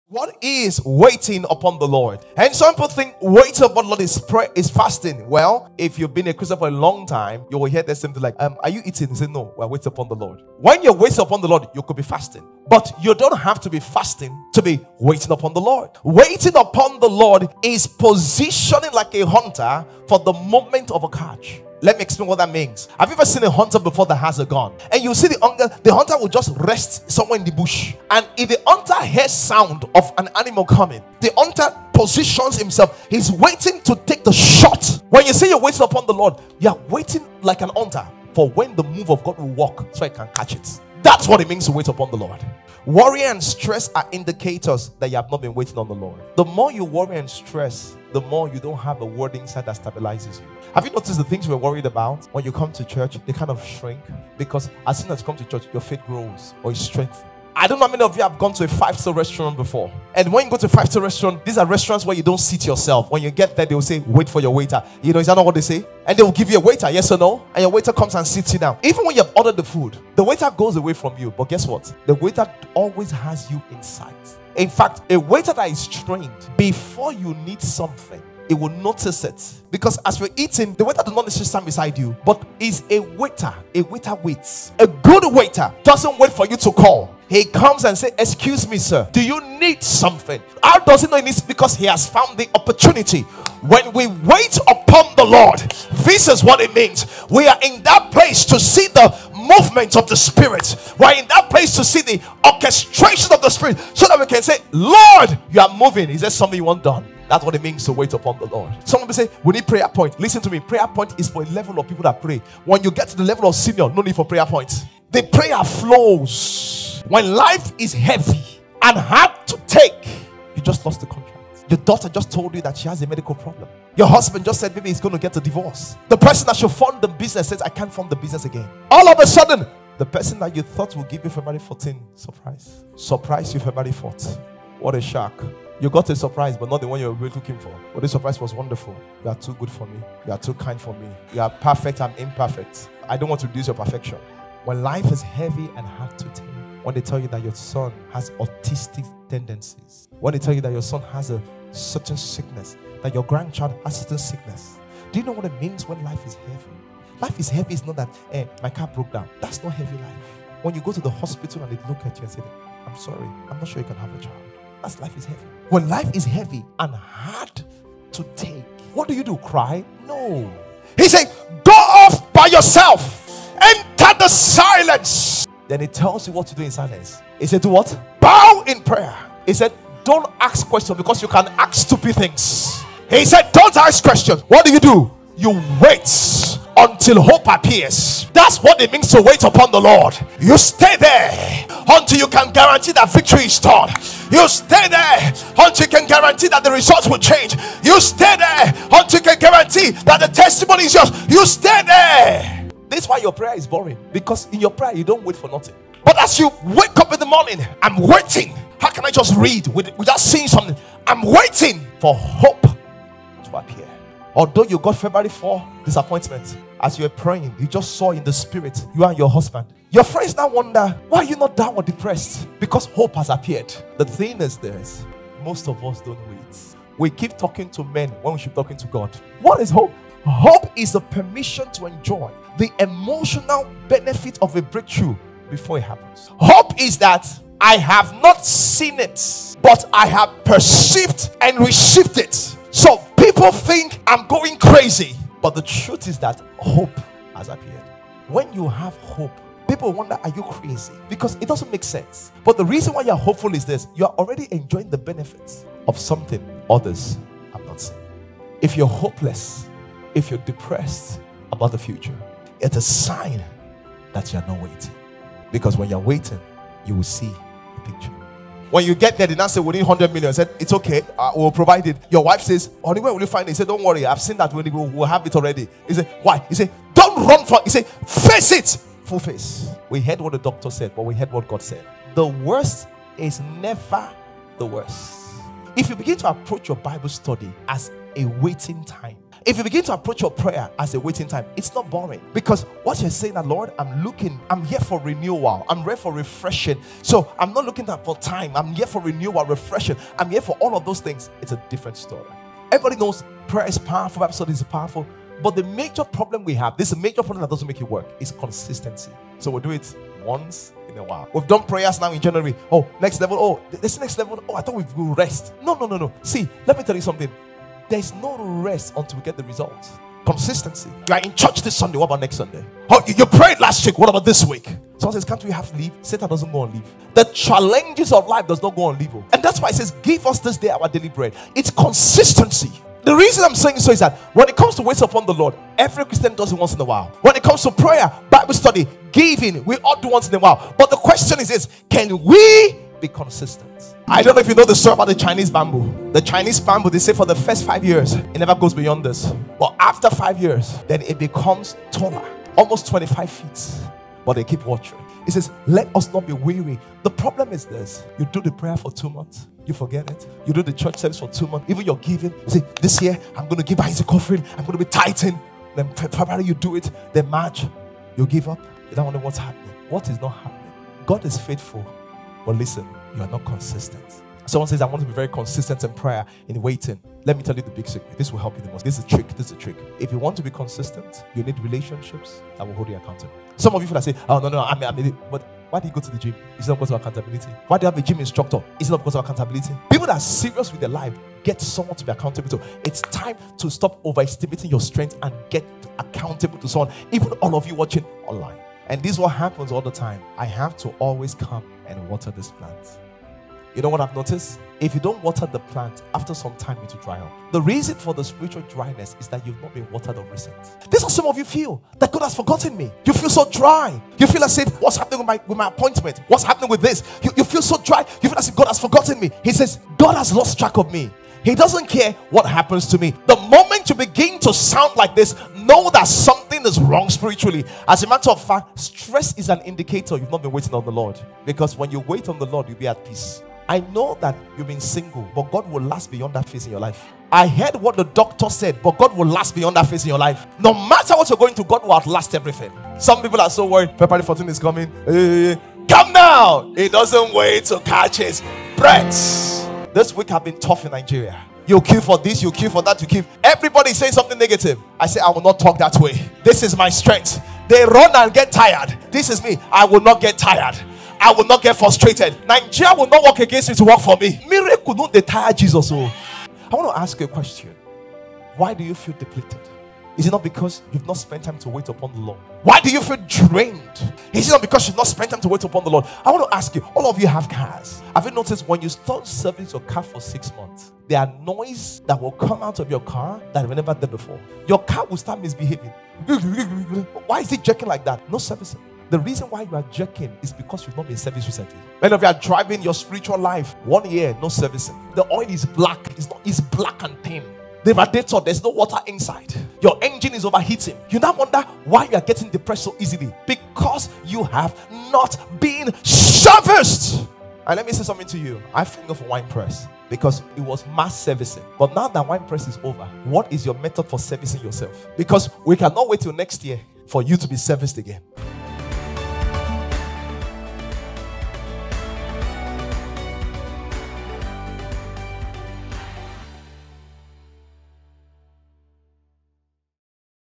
How To Position To Hear From God | Pastor Bolaji Idowu
This is an excerpt from "How To Have A Rich Prayer And Bible Study Time” To watch the full message from @Harvesters,